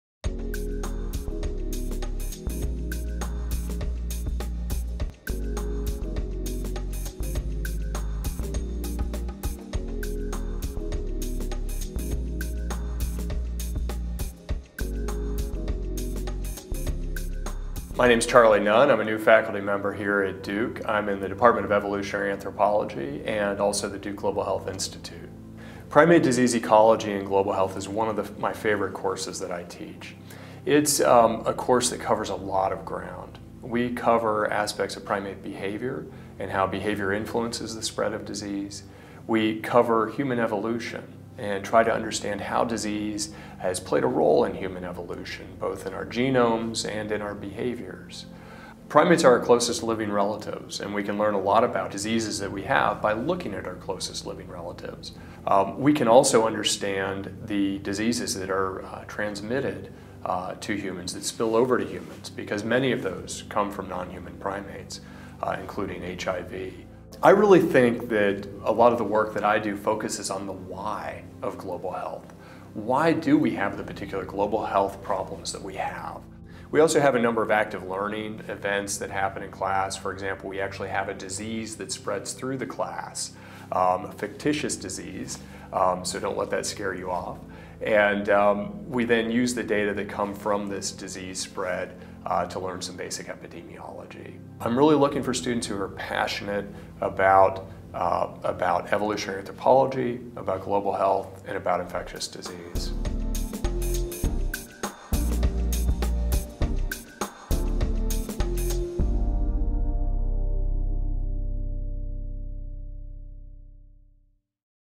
New Course: Primate Disease Ecology and Global Health
Dr. Charles Nunn, a new faculty member at Duke and the Duke Global Health Institute, will teach this new undergraduate course at Duke in spring 2014. A first of its kind to be offered at Duke, the course brings together aspects of global health, evolutionary anthropology and infectious diseases. (EVANTH 385D/GLHLTH 315D counts toward the global health major as a Focused Study and the minor as an Elective.)